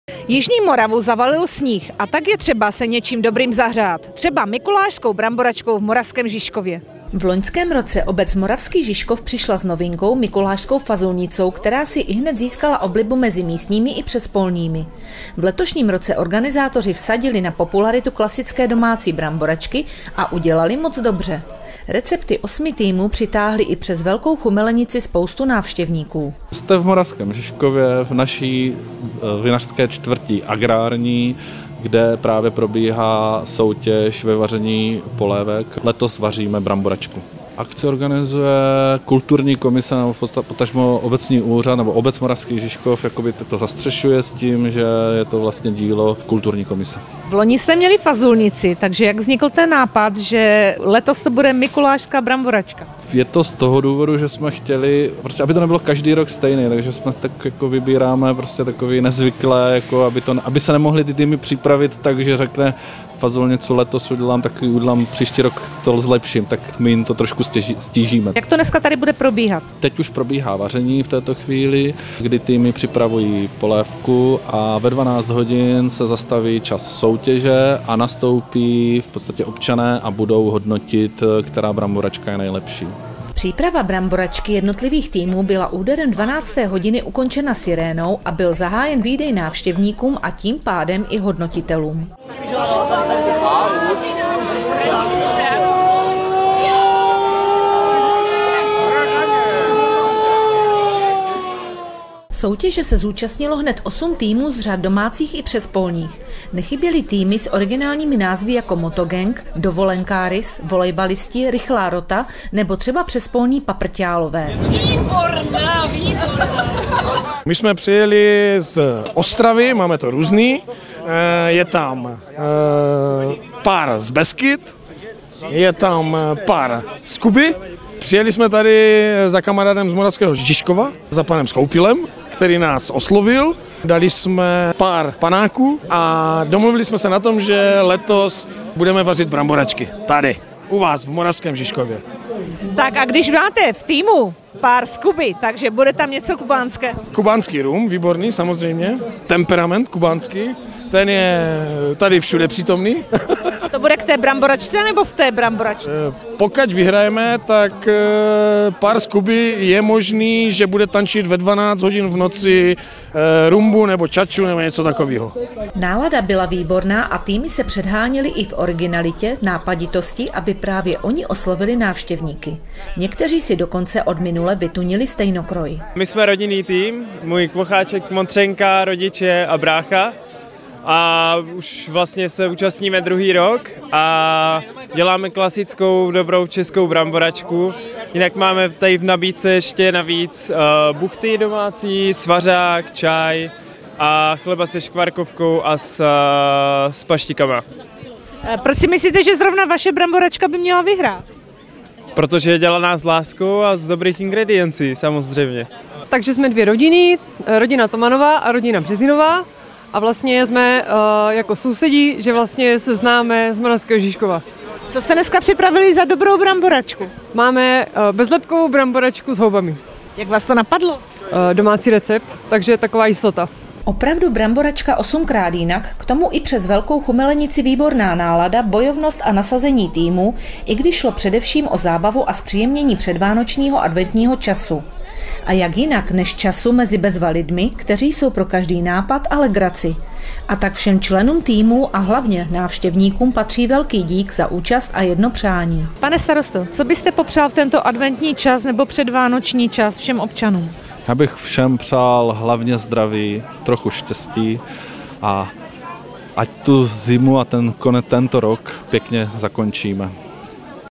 V Moravském Žižkově se zahřívali Mikulášskou bramboračkou
V loňském roce obec Moravský Žižkov přišla s novinkou „Mikulášskou fazulnicou“, která si ihned získala oblibu mezi místními i přespolními. V letošním roce organizátoři vsadili na popularitu klasické domácí bramboračky, a udělali moc dobře. Recepty 8-mi týmů přitáhly i přes velkou chumelenici spoustu návštěvníků.